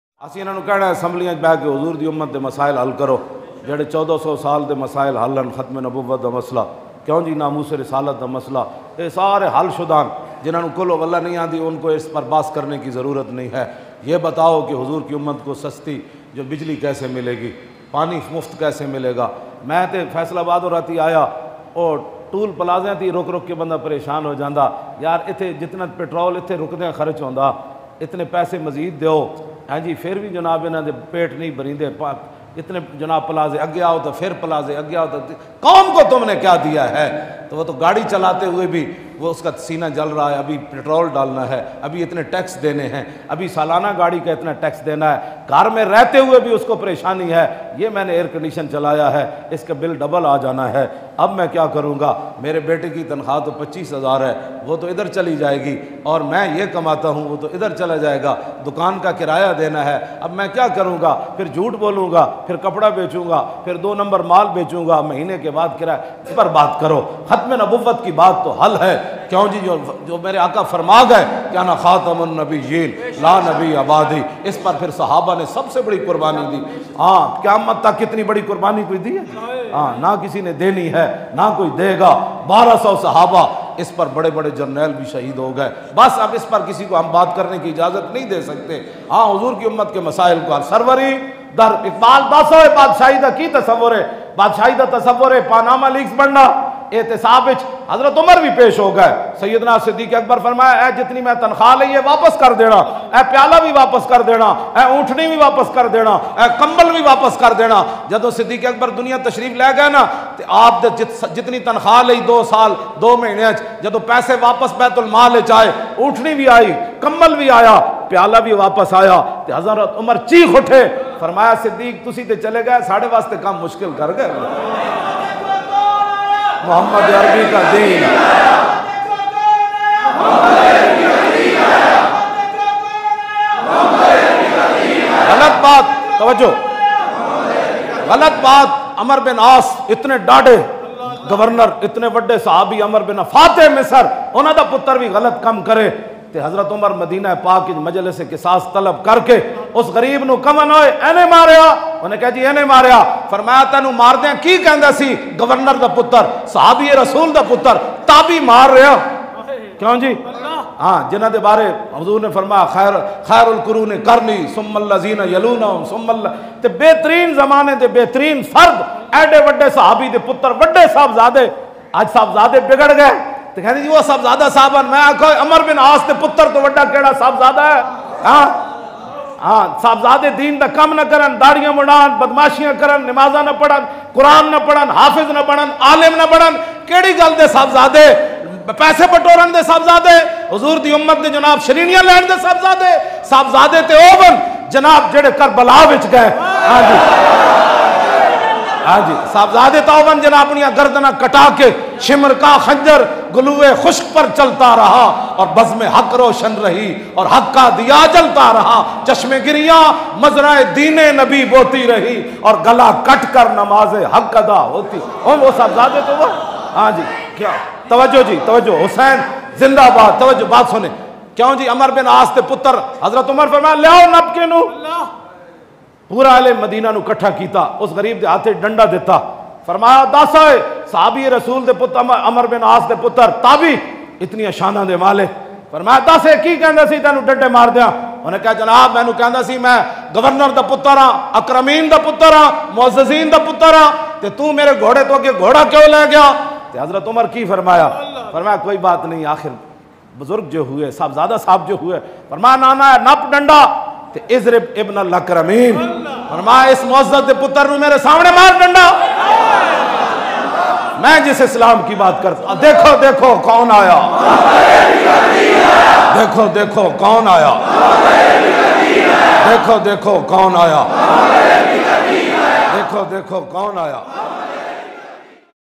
Allama Khadim Hussain Rizvi What is Kingship in Islam HD Latest Bayan With English Subtitle
This video is related to Allama Khadim Hussain Rizvi's speech in which he describes about the rules of Islam this video is not against any religion. This is for Muslims who don't have so much awareness about islam